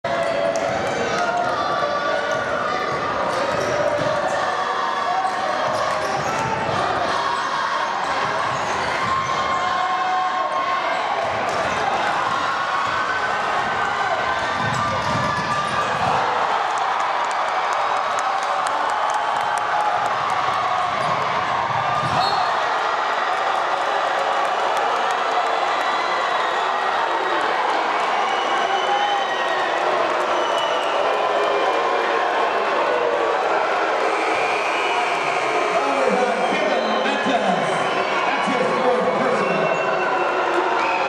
Boys CA high school D4 north section finals: Two blocked shots and a charge
As the third quarter comes to an end Gridley's Kevin Mattos blocks Johnny Saephon's shot, recovers the ball, and takes it coast to coast for a layup, only to be called for a questionable charge foul.